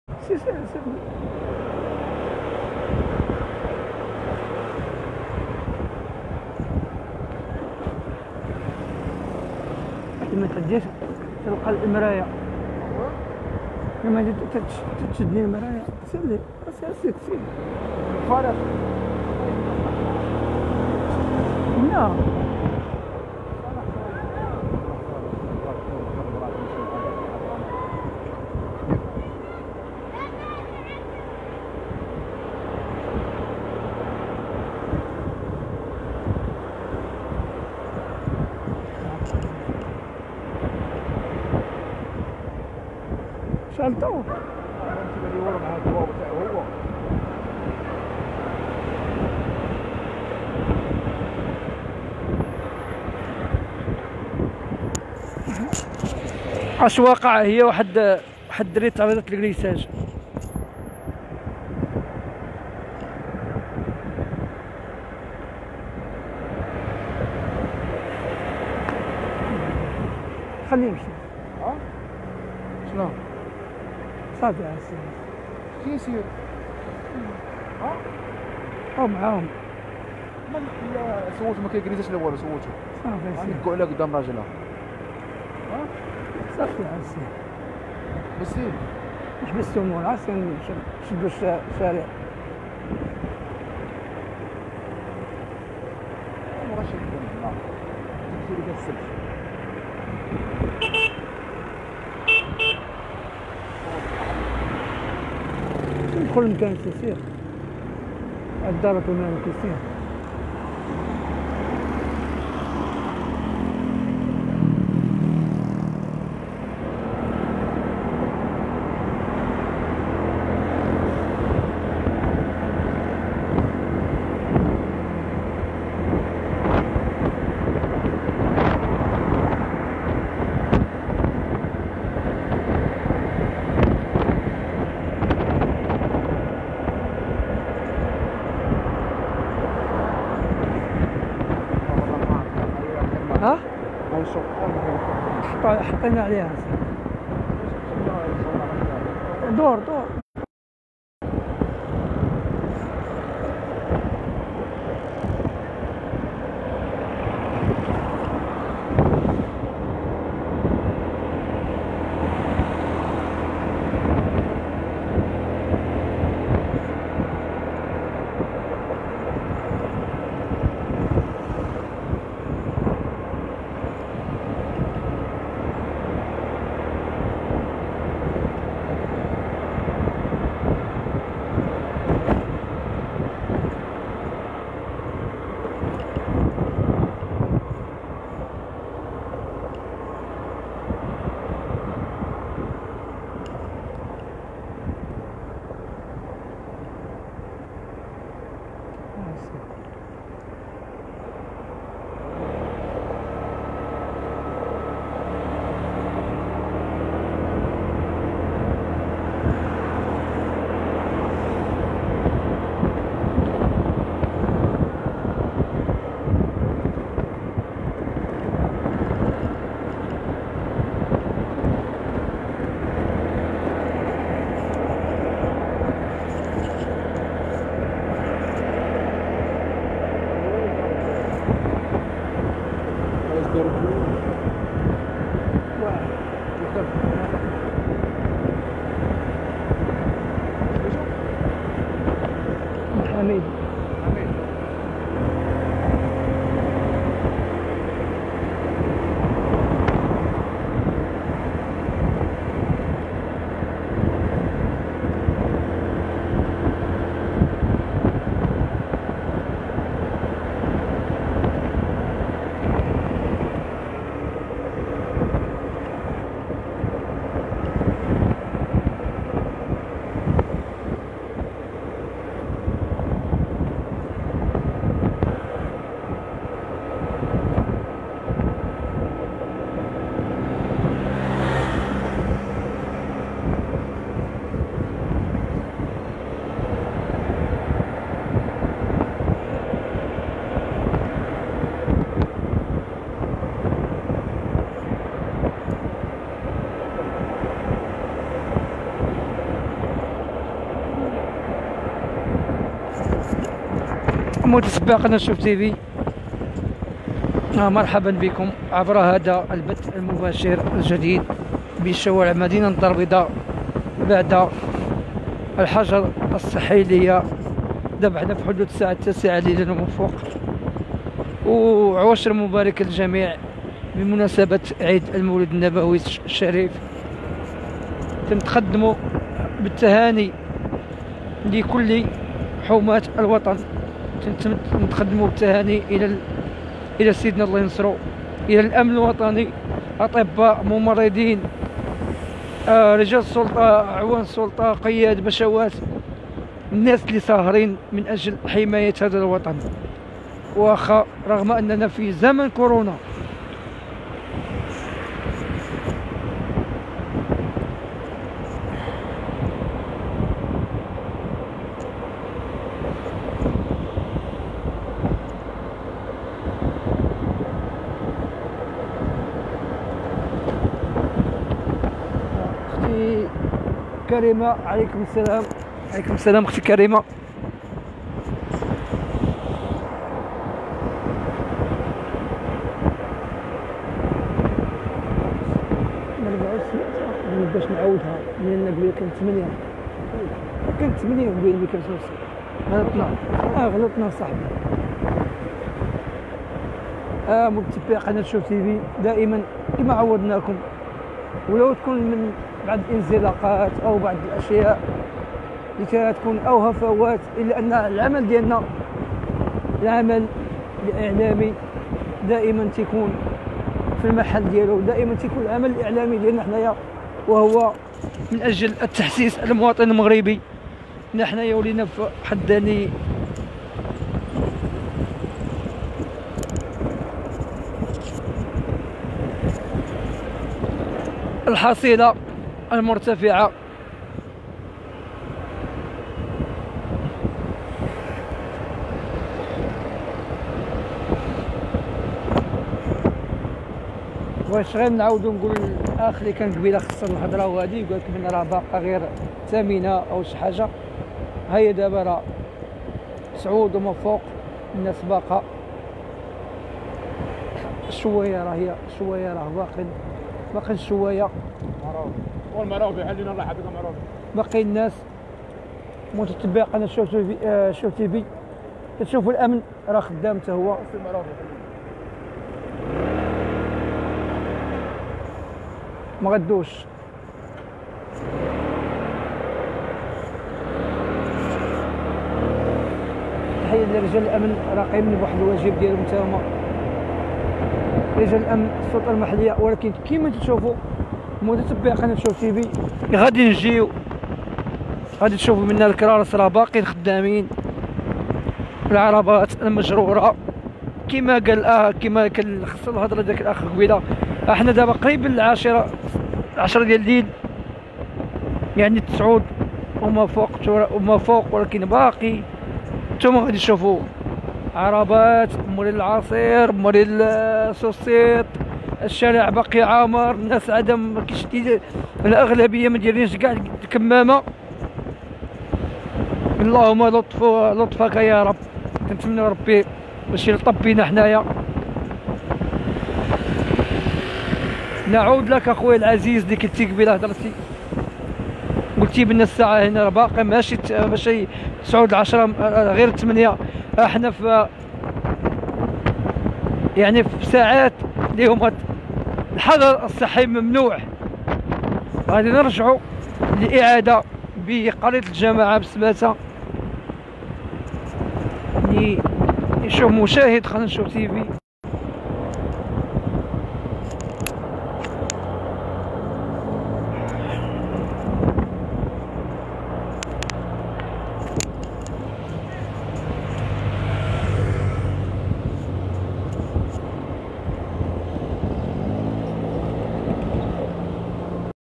من الدارالبيضاء:أجواء ليلية مثيرة بشوارع وأحياء كازا بمناسبة ذكرى المولد النبوي والناس باقين خارجين
مباشرة من الدارالبيضاء:أجواء ليلية مثيرة بشوارع وأحياء كازا بمناسبة ذكرى المولد النبوي والناس باقين خارجين